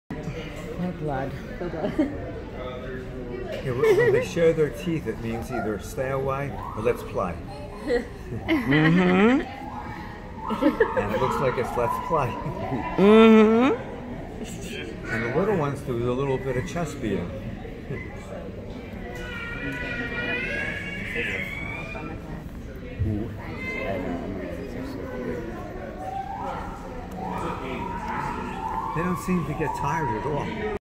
Baby gorilla wrestlemania!
3 year old AMANI  plays with her 2 year old half brother AJABU @the Philadelphia zoo